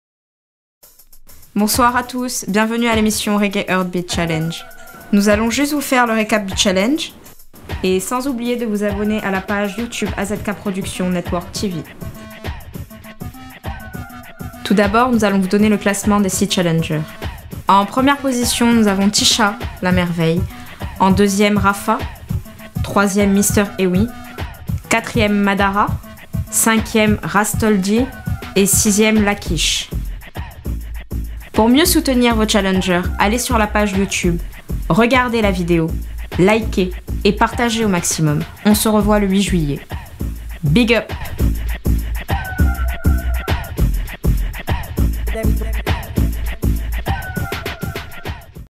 RECAP REGGAE HEARTBEAT CHALLENGE by AZKproductions
Bonsoir amis

Petit récap sur le reggaeheartbeatchallenge avant la finale, nous avons donc 6 chalengeurs bien déterminés.

⚠️ DERNIÈRE LIGNE DROITE ⚠️ IL FAUT DONC ÊTRE AU MAXIMUM DERRIÈRE VOTRE POULAIN COMME ON DIT; À VOUS DE TOUT D’ABORD VOUS ABONNEZ SUR LA CHAÎNE YOUTUBE , PUIS REGARDEZ LA VIDÉO DE CELUI-CI, ENSUITE LIKEZ ET ENFIN PARTAGEZ AU MAXIMUM ⏳

BIG UP ❤️💛💚👊🏽